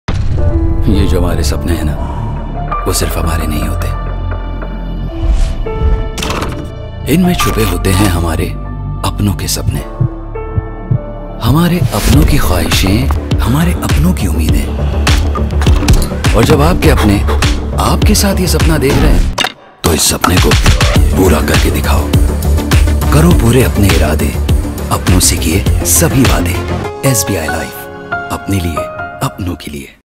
Turning Dreams into Reality | Apne Liye. Apno Ke Liye.
This cricket season, be a Responsible Passion Chaser and fearlessly chase your dreams while financially securing your loved ones.